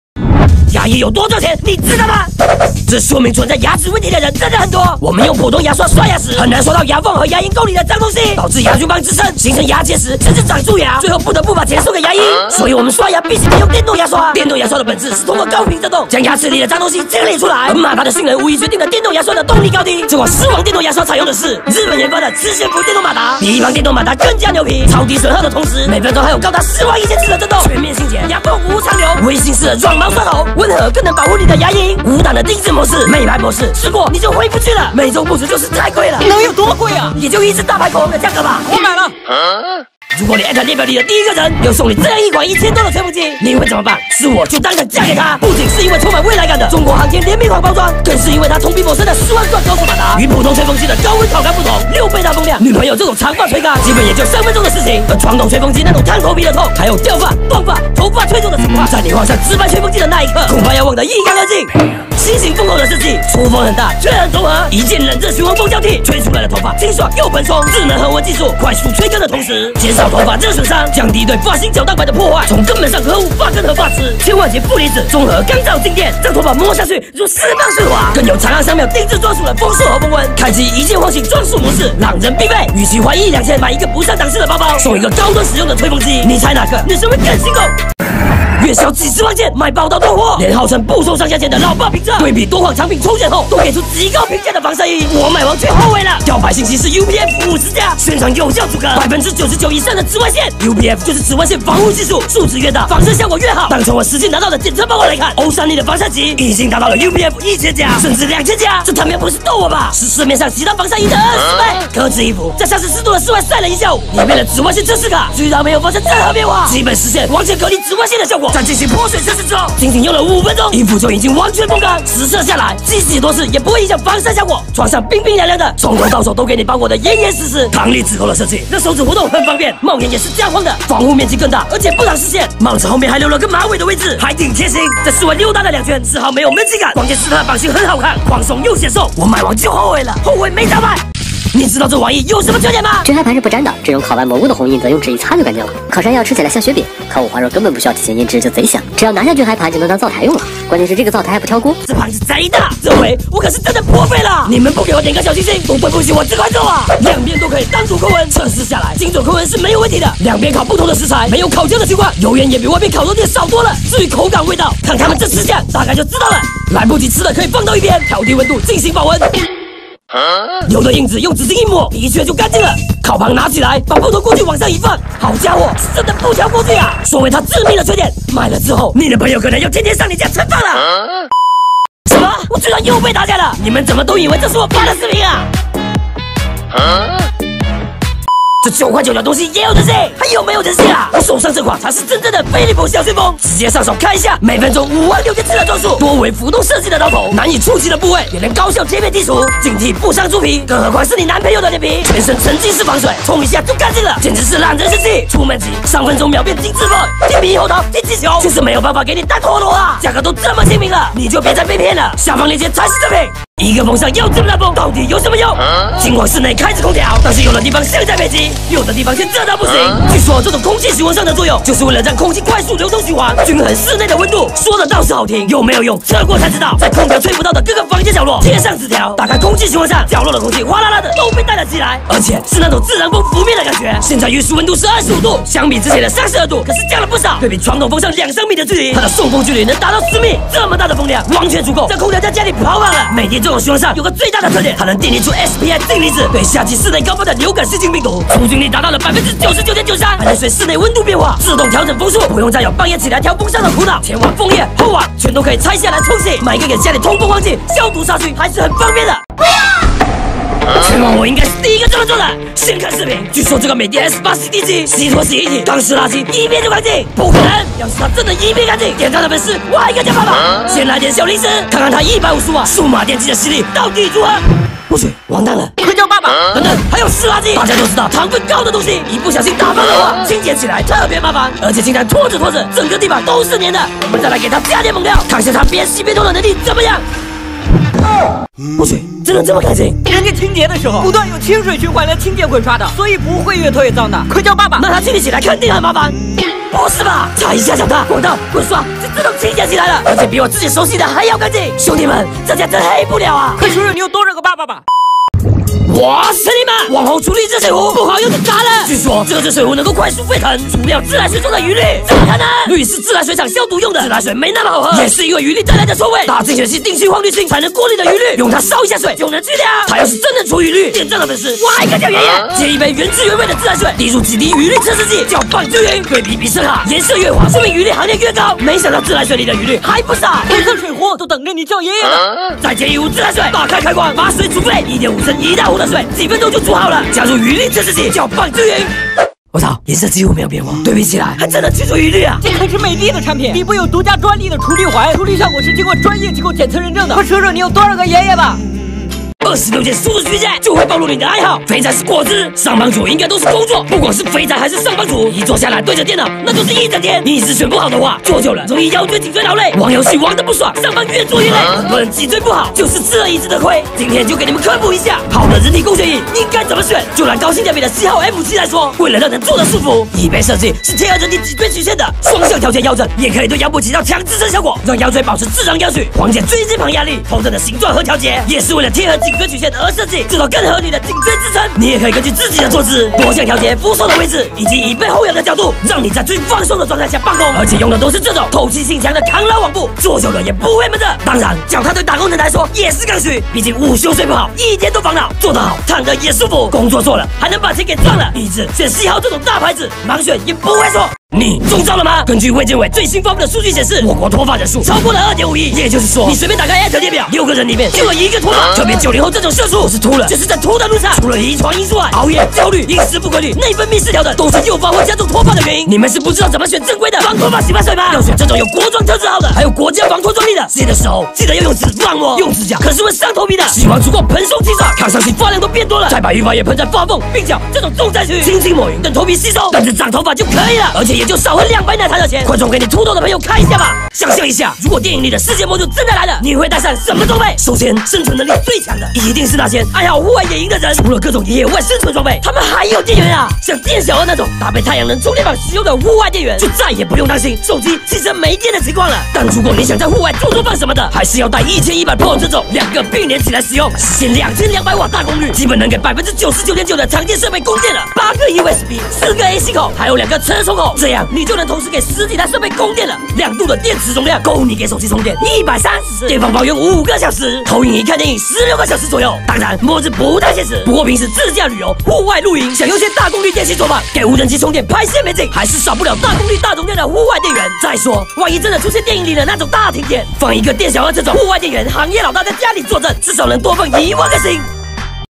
【不好用就砸】直白吹风机测评，洗地机大测评，人体工学椅怎么选
00:00 磁悬浮电动牙刷
00:39 直白吹风机测评
01:27 防晒衣测评
02:21 多功能料理机测评
03:01 男士剃须刀测评
03:33 空气循环扇测评
04:19 洗地机大测评
05:05 网红除氯壶测评
05:49 人体工学椅怎么选
06:41 防脱洗发水测评
07:14 末日必备生存装备